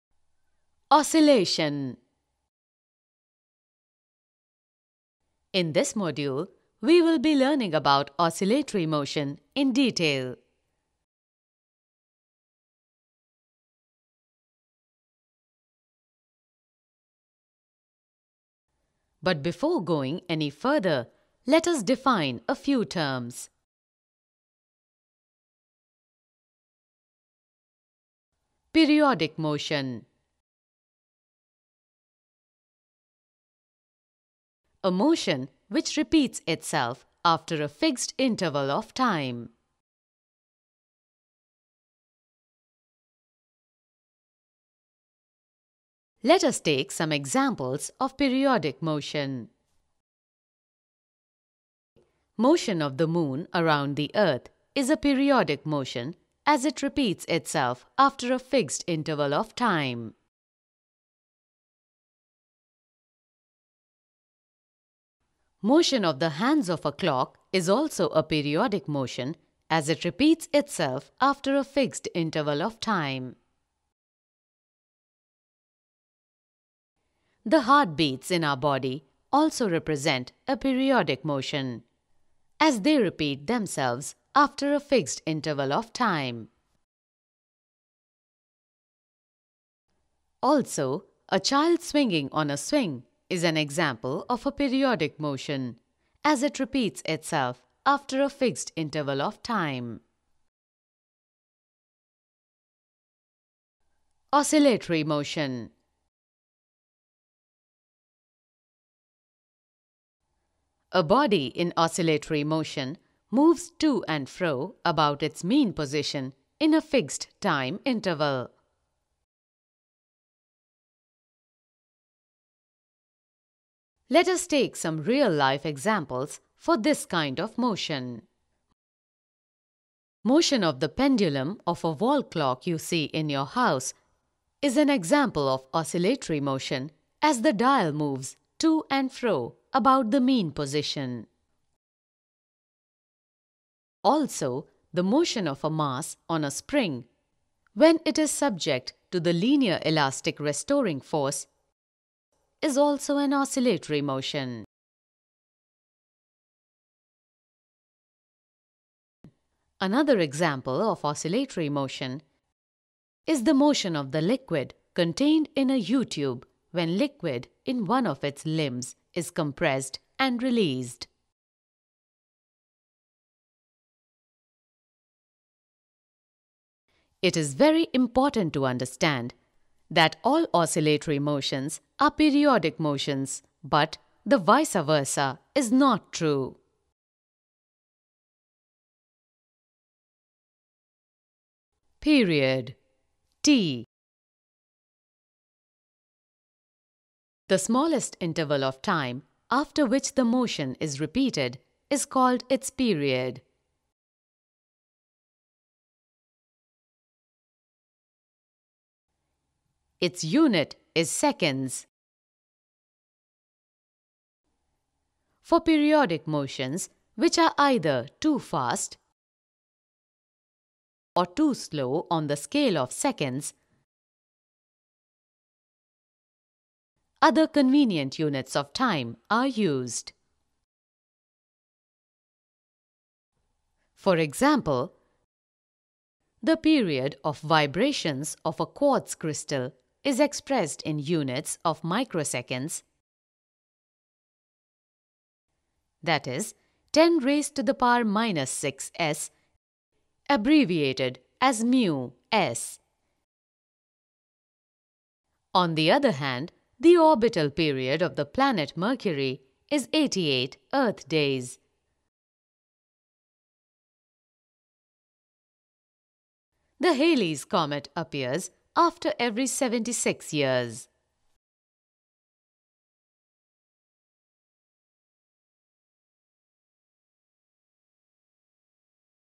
Oscillation | Physics | Oscillatory Motion | Periodic Motion
Periodic motion-  A Motion that Repeats itself after a Fixed interval of Time, Motion of a Hands of a Clock is also a Periodic Motion. The Heart Beats also Represent Periodic Motion, A child Swinging on a swing.
Oscillatory Motion- A body in Oscillatory Motion moves to and fro about its means position in a fixed time interval. Example - Pendulum

All Oscillatory Motion are Periodic motion but all periodic motion is not oscillatory motion.

Learn About Representation of Period (T)

For the Periodic Motion which is Too Fast or Too slow on the scale of seconds, an alternative convenient unit of time is used. Watch to Know how we measure the period of Vibrations of a quartz Crystal which is expressed in units of Microseconds.